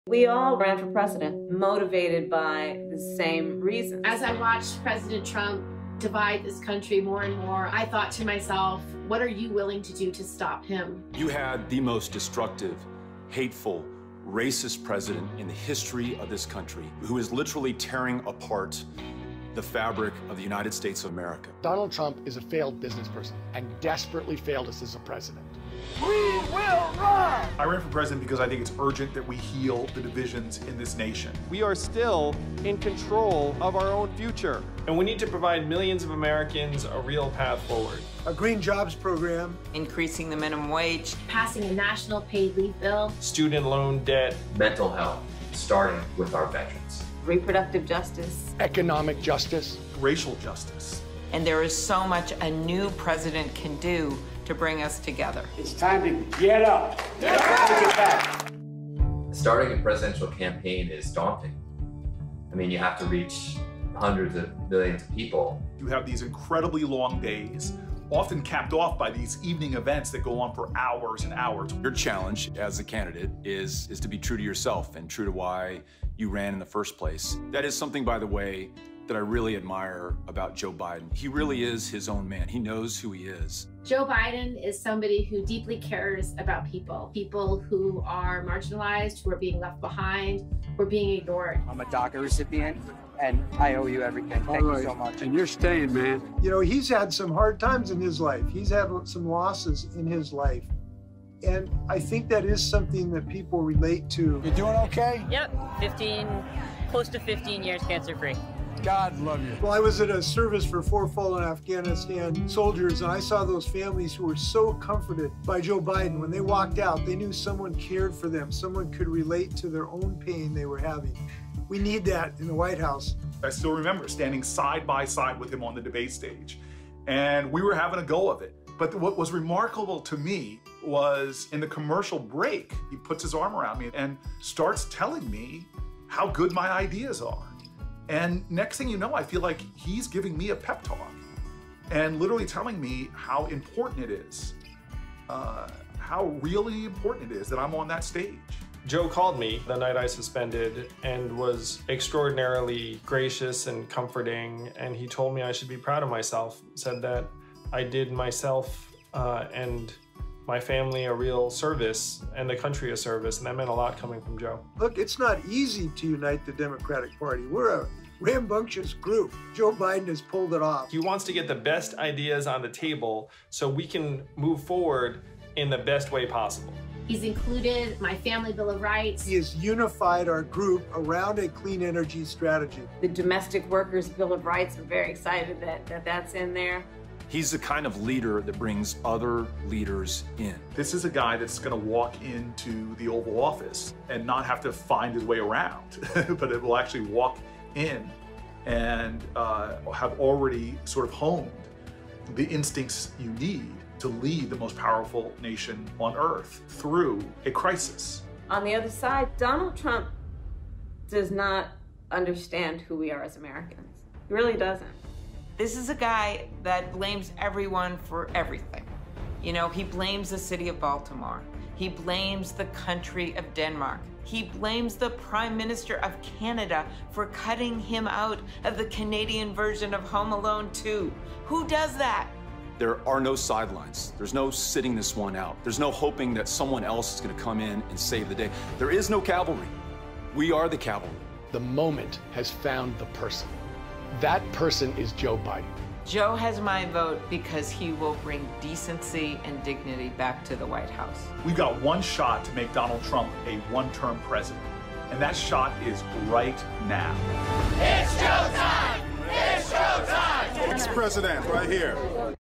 Former Democratic Presidential Candidates Back Biden at the DNC | NowThis
It’s Joe time’ — Former Democratic primary candidates banded together to support Joe Biden on the first night of the DNC.